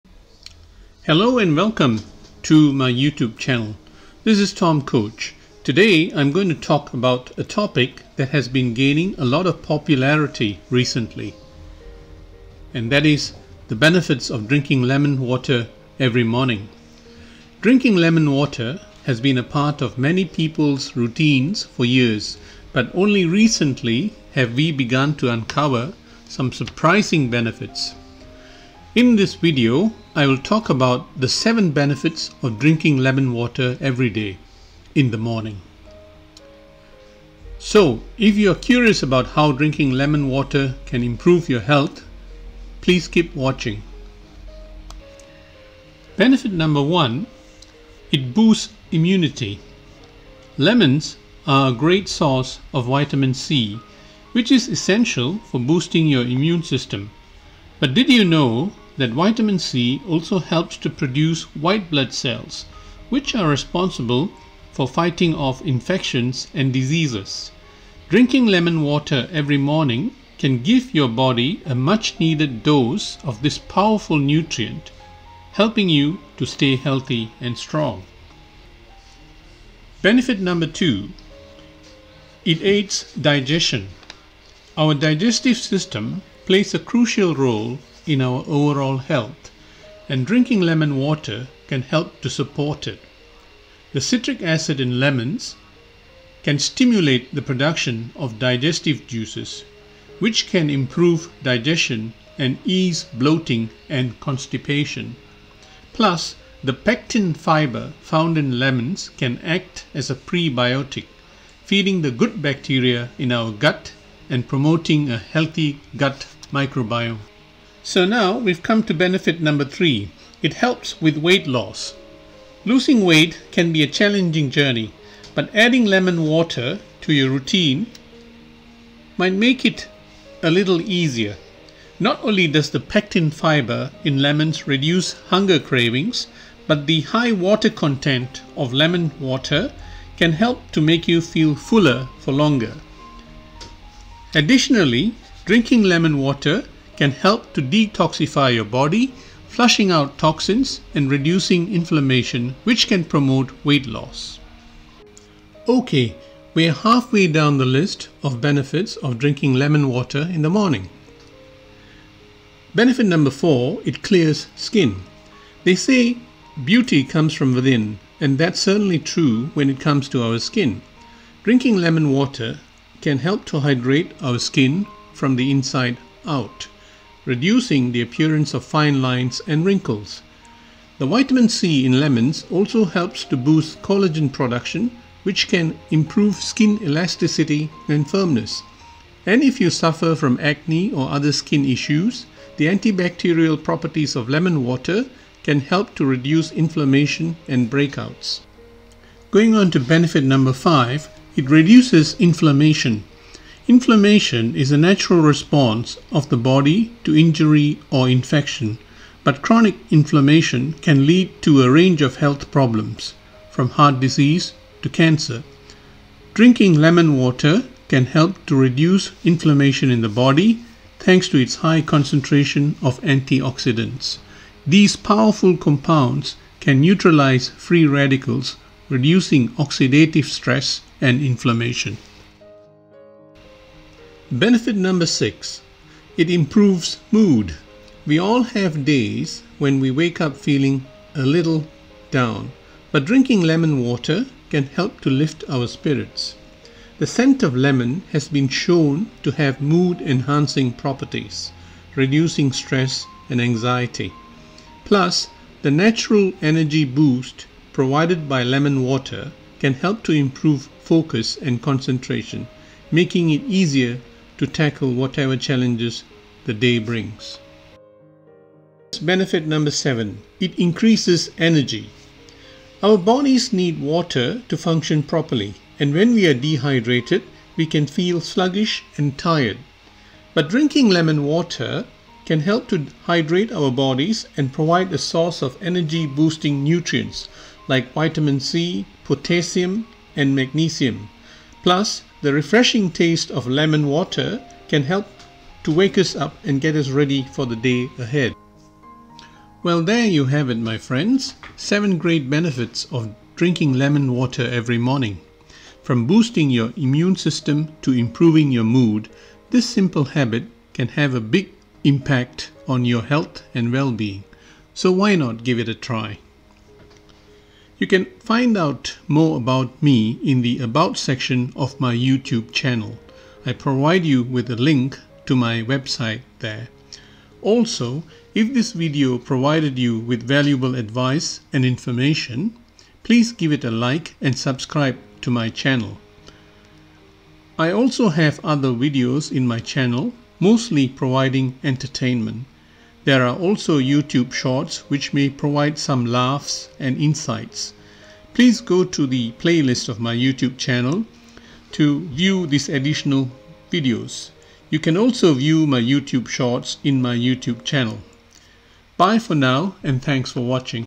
7 Benefits Of Drinking Lemon Water Every Morning
Hey There. Here Is Something About Lemons And Drinking Lemon Water That May Interest You. Lemon Is A Power House Fruit, Packed With A Lot Of Surprises Which Can Improve Your Health And Well-Being.